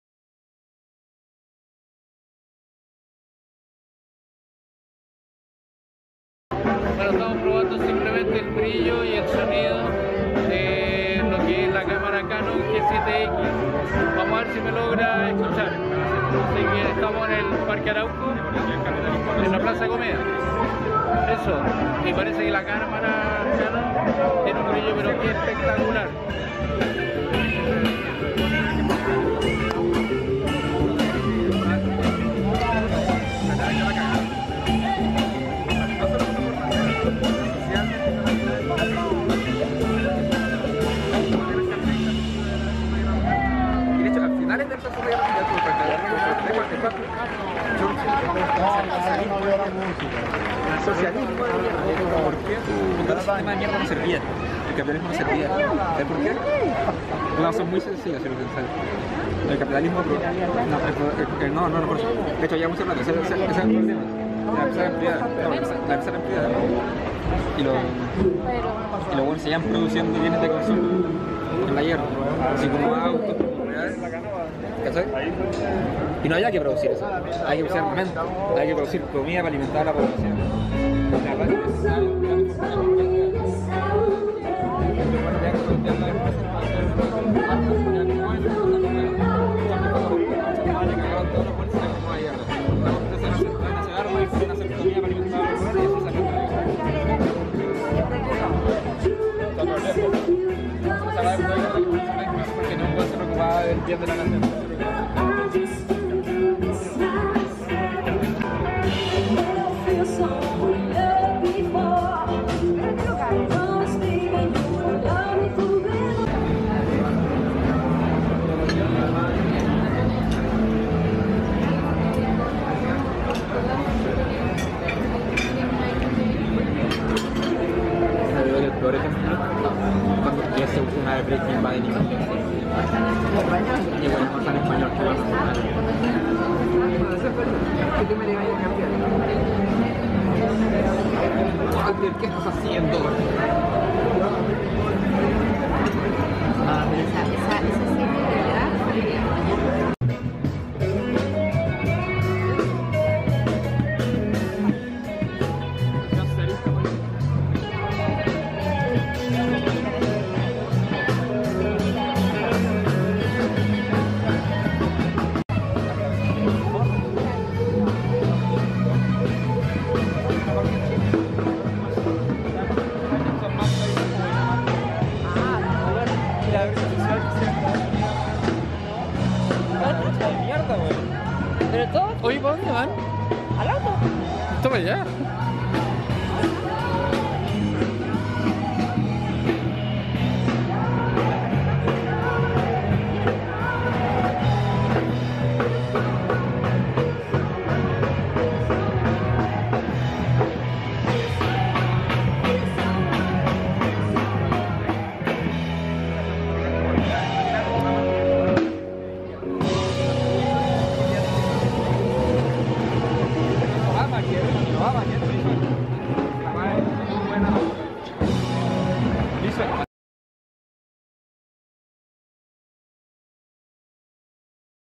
Canon G7X Night Test
Es una simple prueba de diversas tomas realizadas durante la noche con la cámara Canon G7X